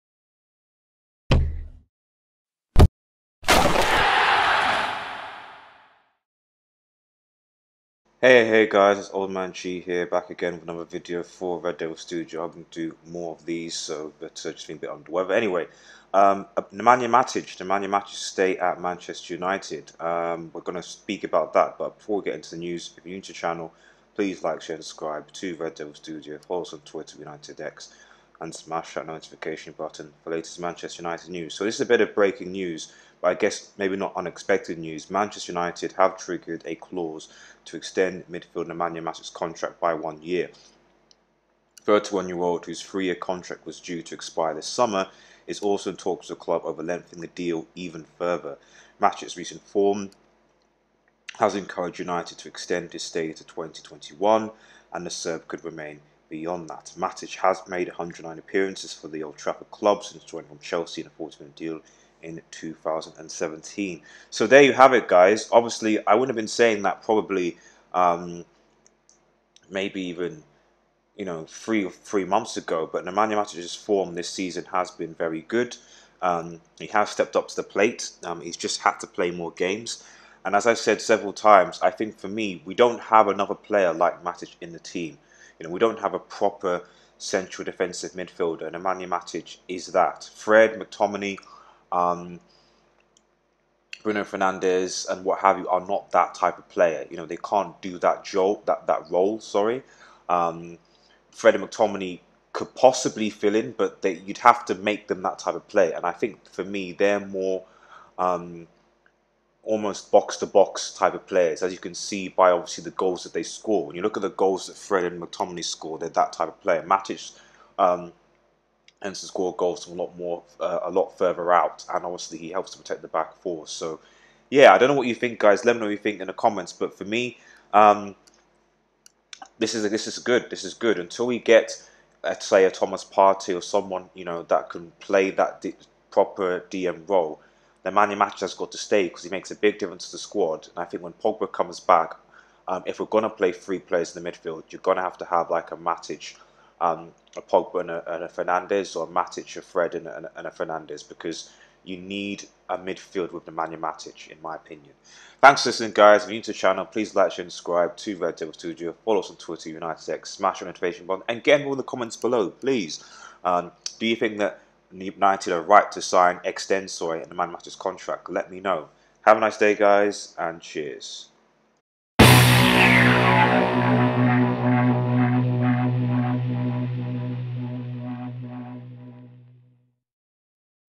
MATIC TO STAY AT UNITED! | LATEST MANCHESTER UNITED NEWS!
It's official United, have opted to trigger an extension on Matic contract for another year! Let me know what you think about this news in the comments!

MATIC TO STAY AT UNITED!